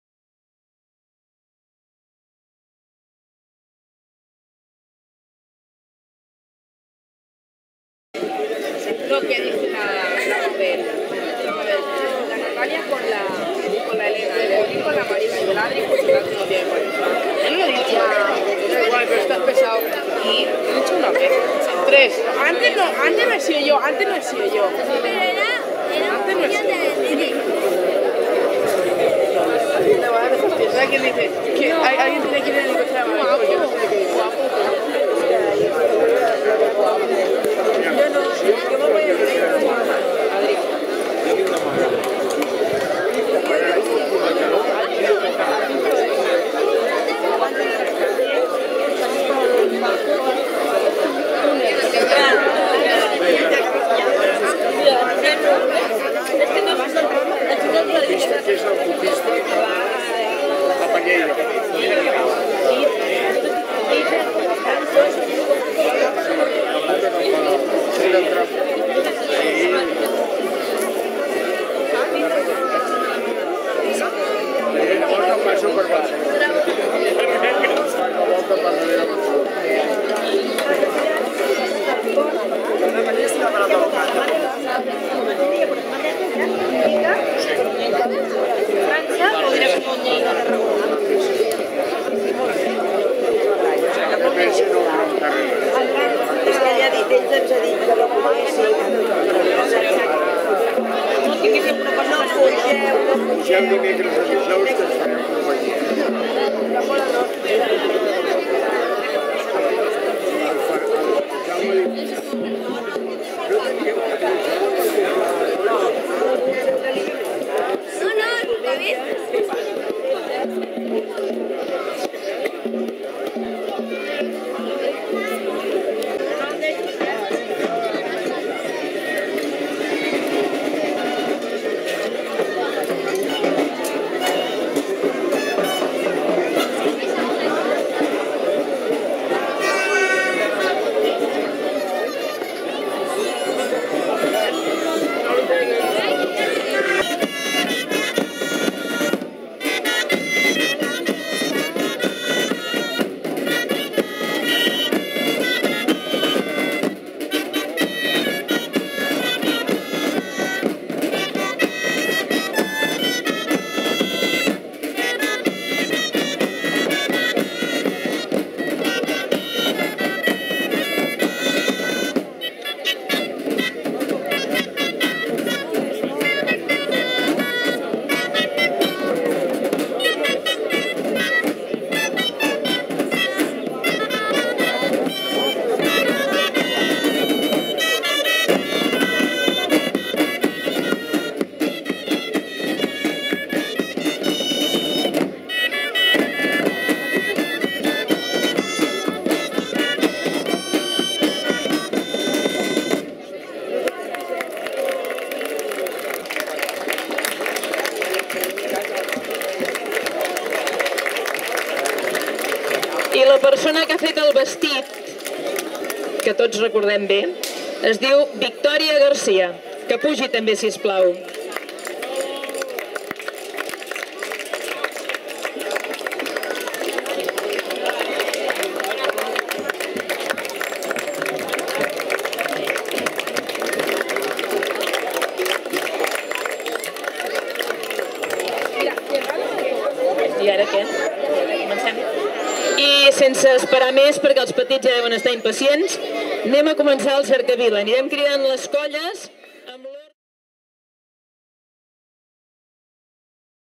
Plantada de Gegants
Festa Major de Vilassar de Mar, plantada de Gegants al Veral de l'Ocata, intervenció del grup SONS, i presentació de la nova figura dels Nans, Roser carrau.